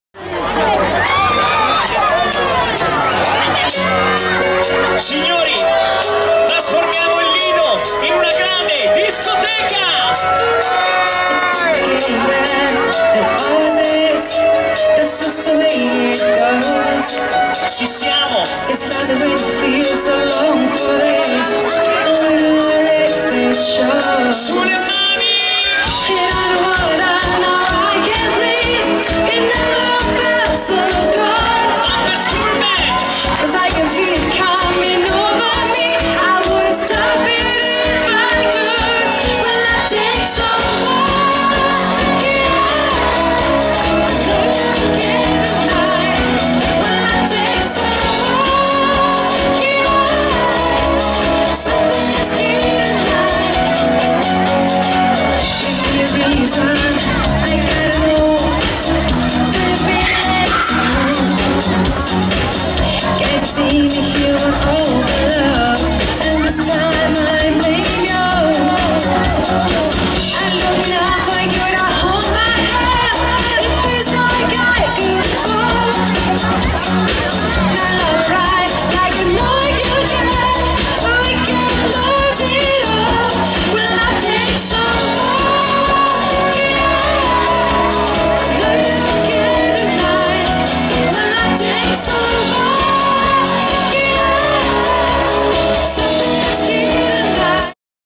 Notte bianca @ Lido Goia - Pisogne BS
Muppets CoverBand live @ Lido Goia - Pisogne BS
Notte Bianca 12 Agosto 2011